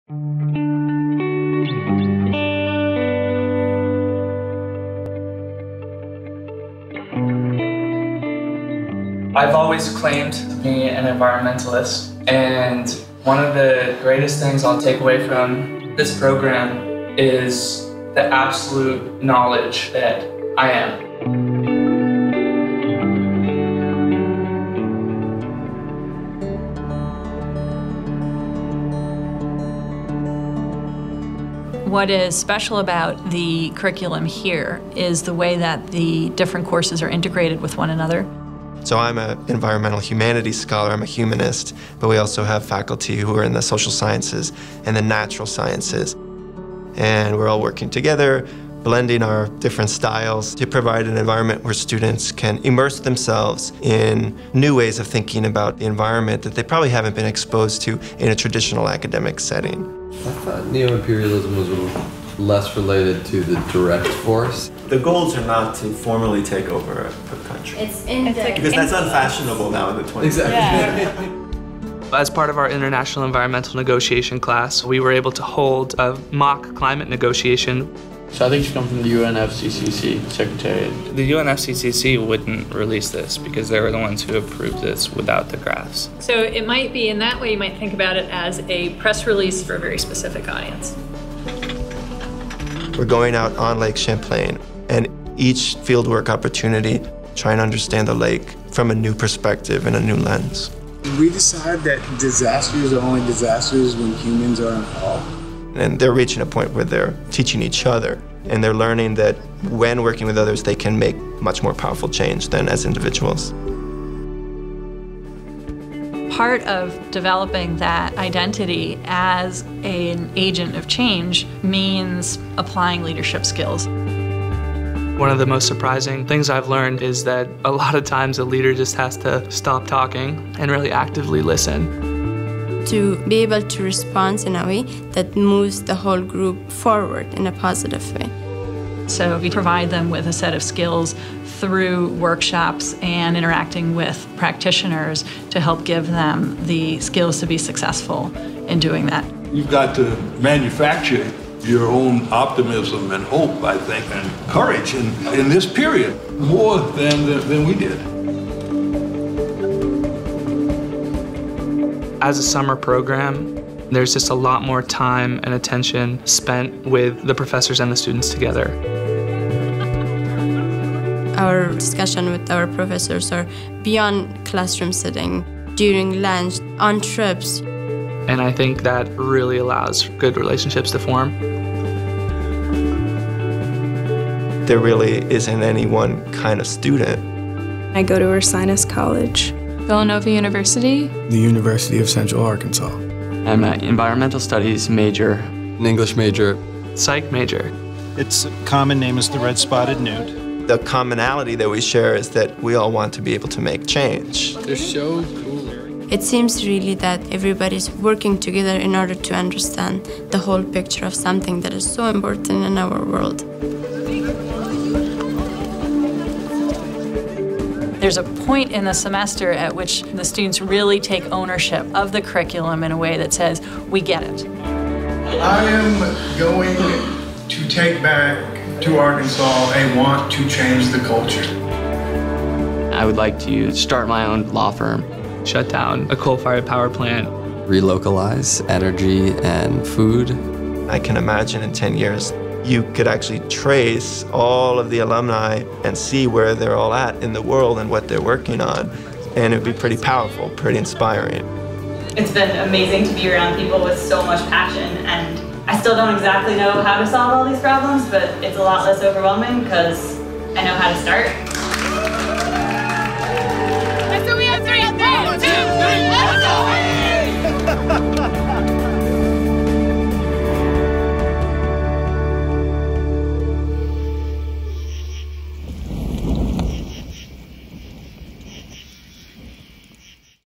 School of the Environment- Middlebury College
Watch this video and learn more about the incredible environmental studies summer program at Middlebury College, Vermont.